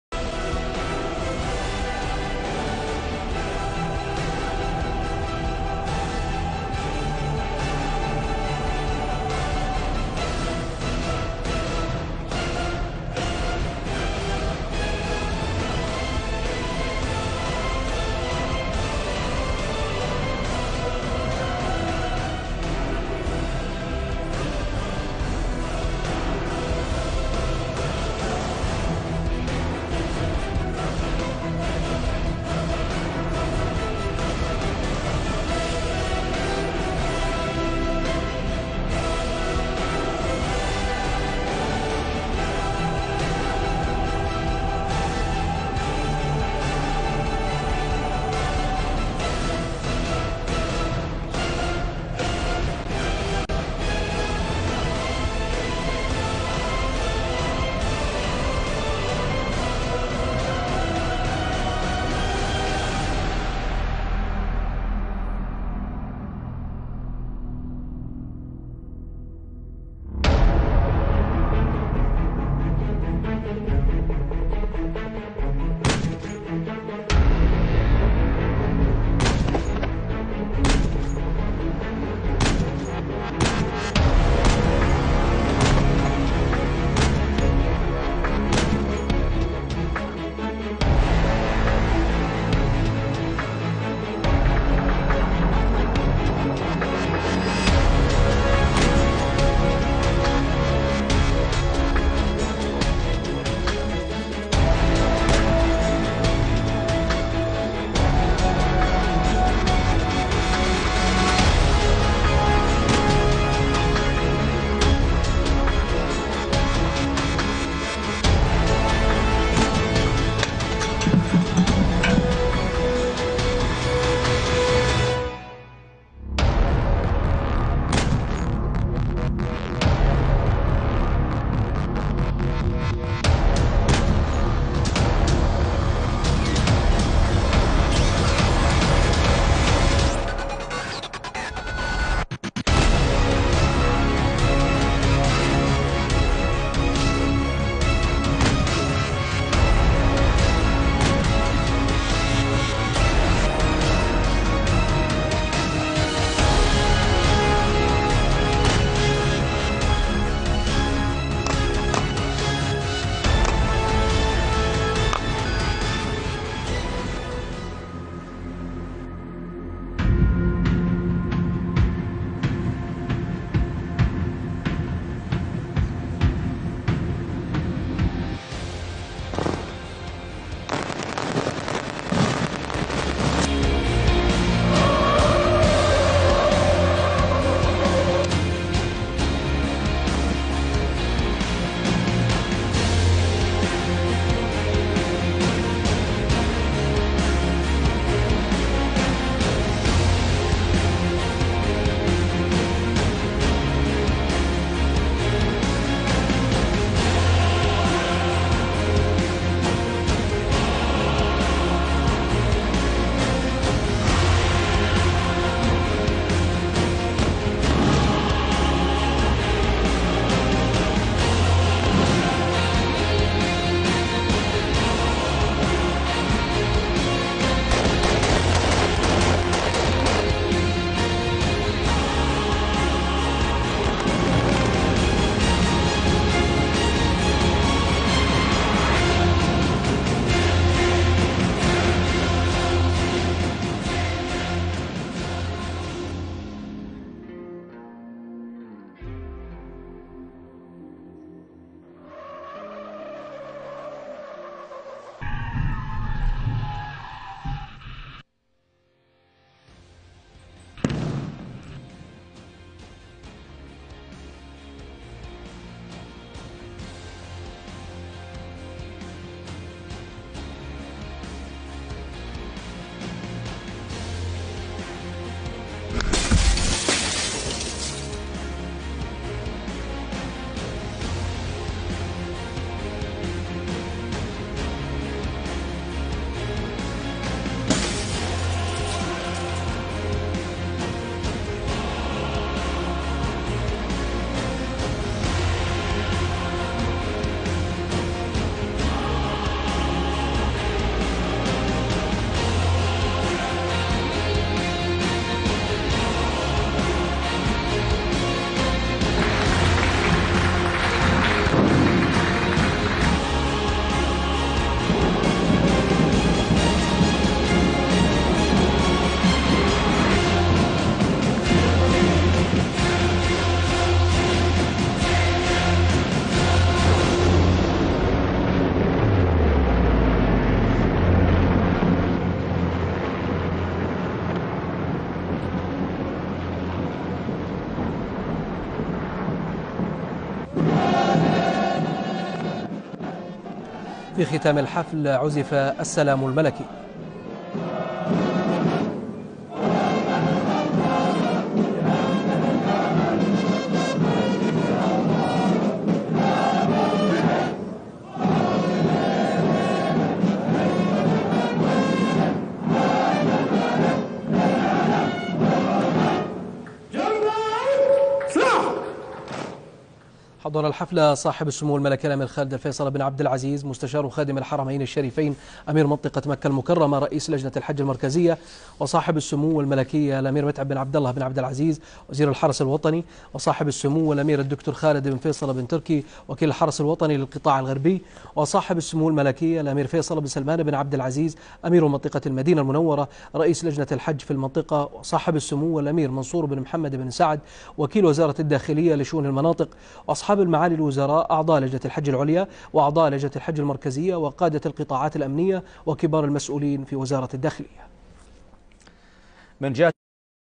Légitime défense
Regarde || La fête annuelle de l'examen des forces de sécurité de pèlerinage "de la deuxième partie" Saudi les forces
Traduit de : Arabes